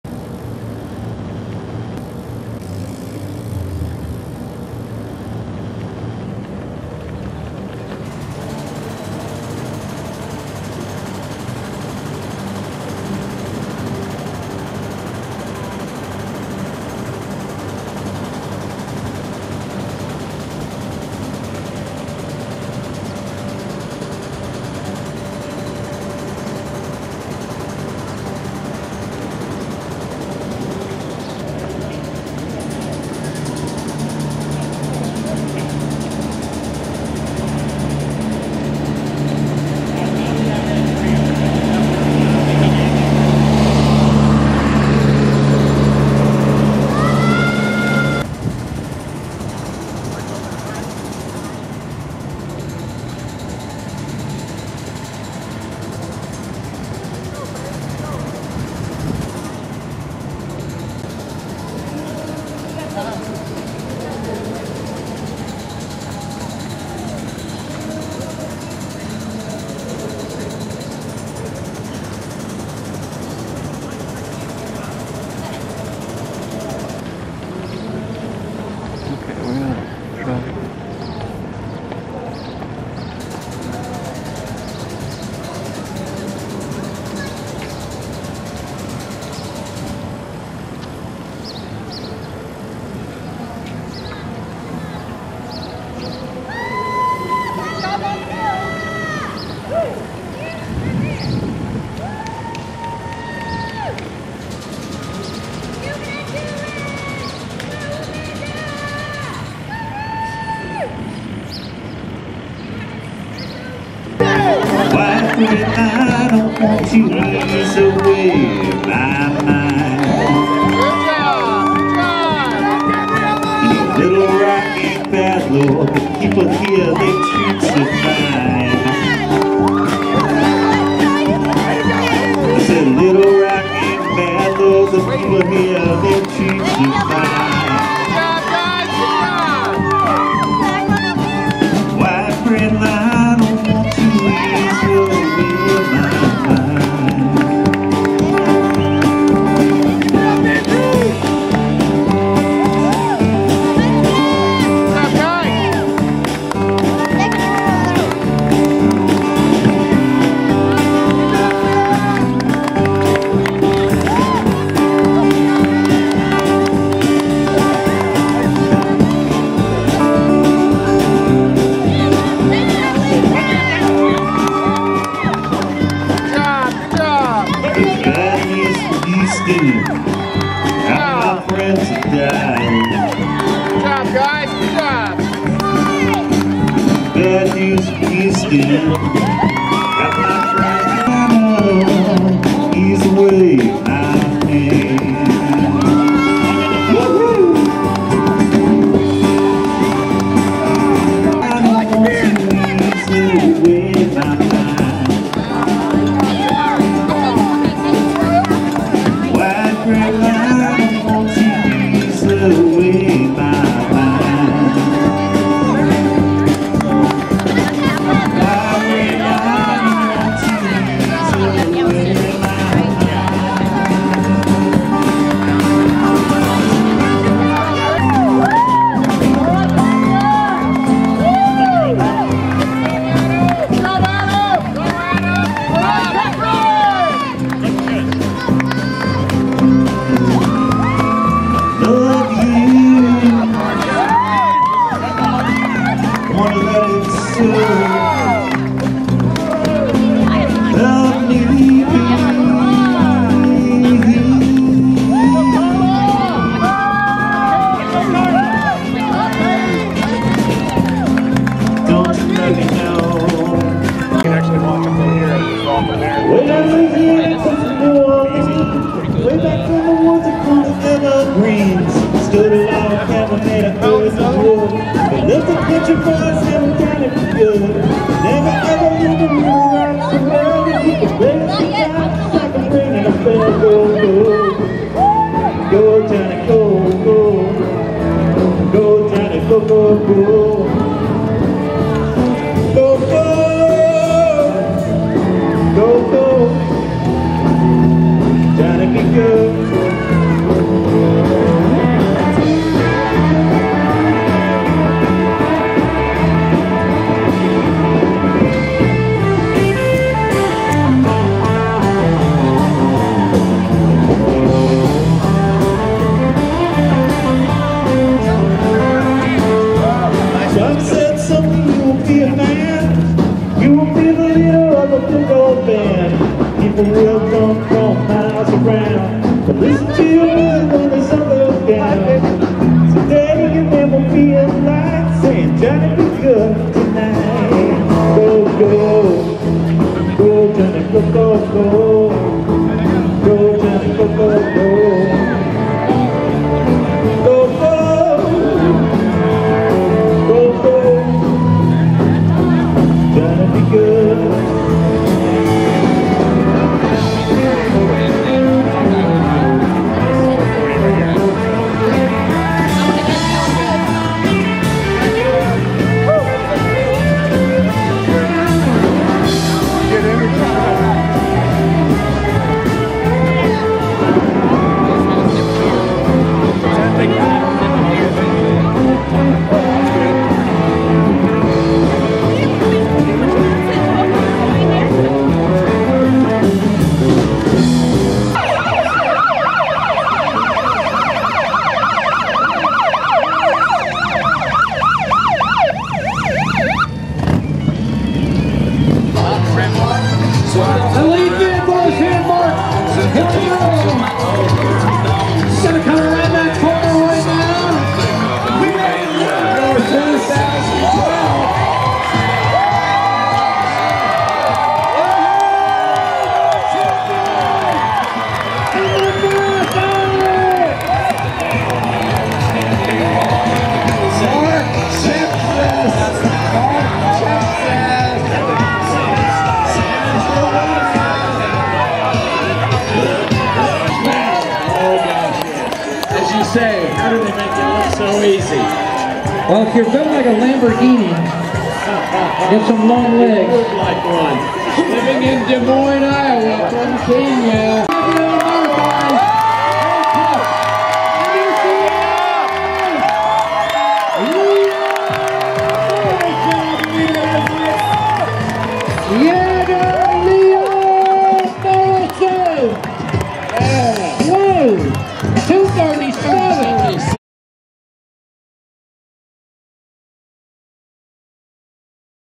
Little Rock Marathon 2012 Capitol
Sharper & Shorter than previous version. Little Rock Marathon mostly on Capitol Street.